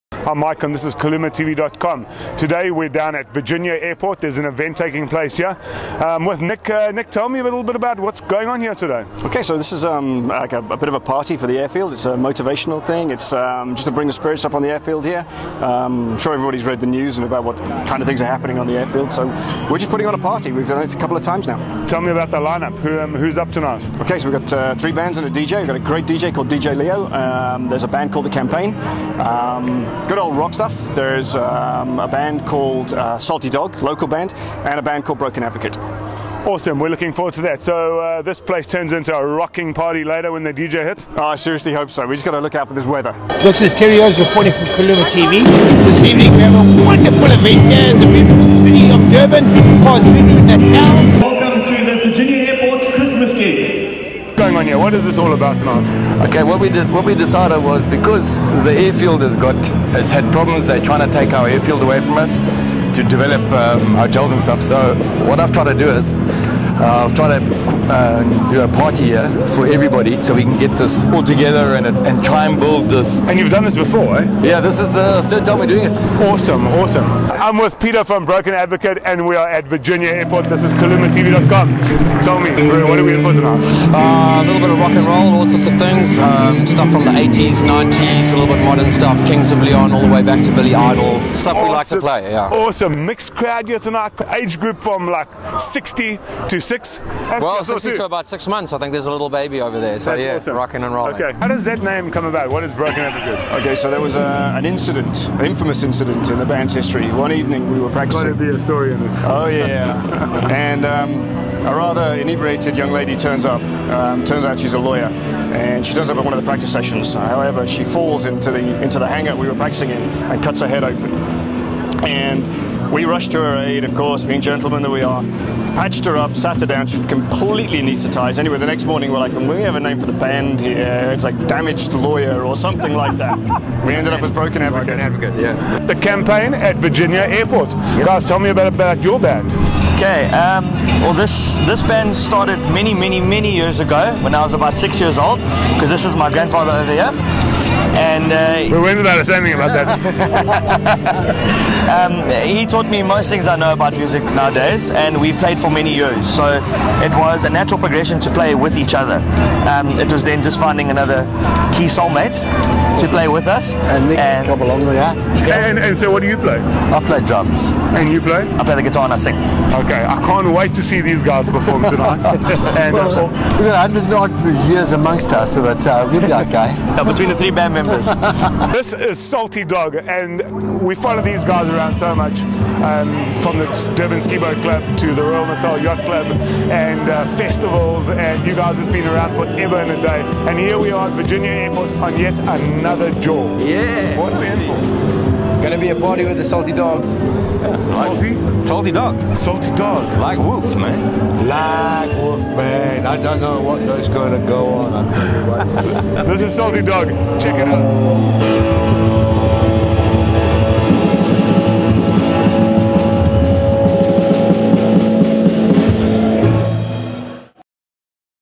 Virginia Airport Gig Durban KZN Dec 2016 Live Music & DJ
The KulumaTv crew went to Virginia Airport on Friday 9 December 2016 to check out some live music. The bands lined up were The event was in aid of uplifting the spirits of air field personnel. A smoking hot underground event held for the third time.